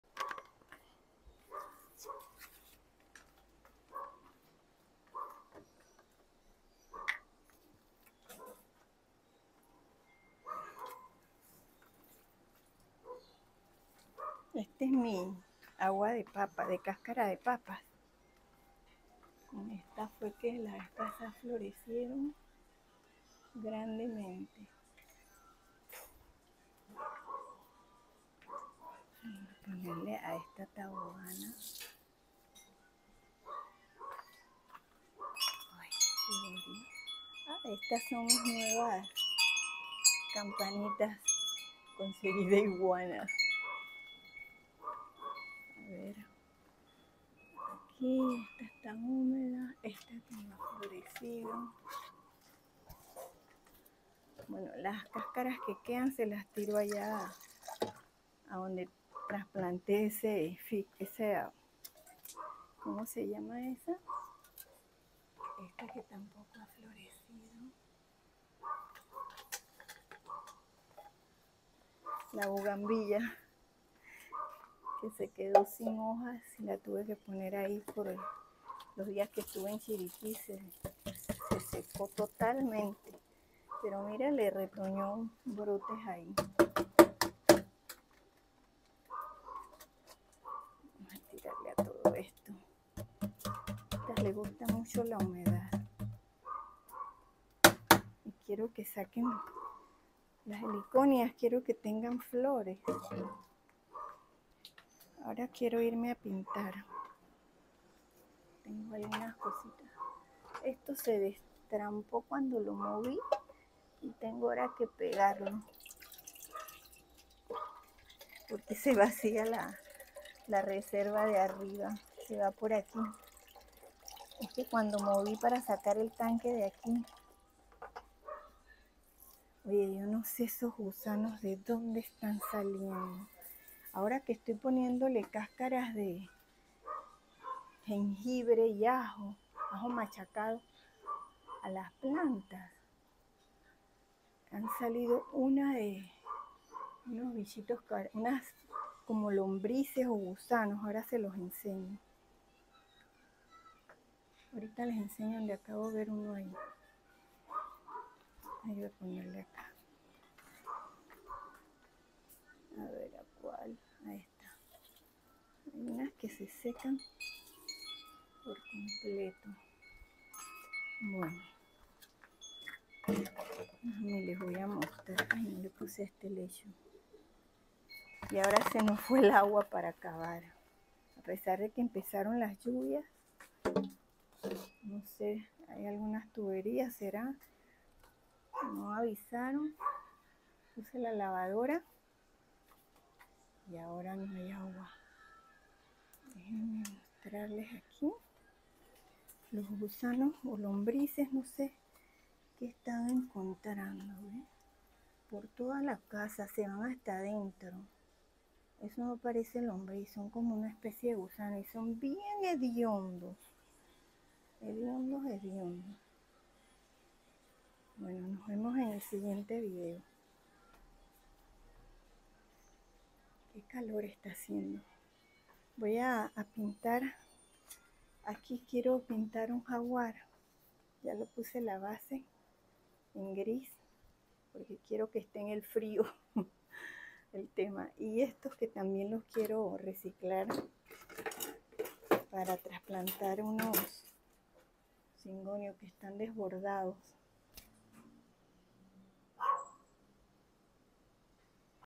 Fertilizando con cascara de papa en agua de arroz
Esta mezcla de agua de arroz y cascara de papa hace florecer porque son ricas en potasio magnesio zinc hierro fósforo vitaminas complejo B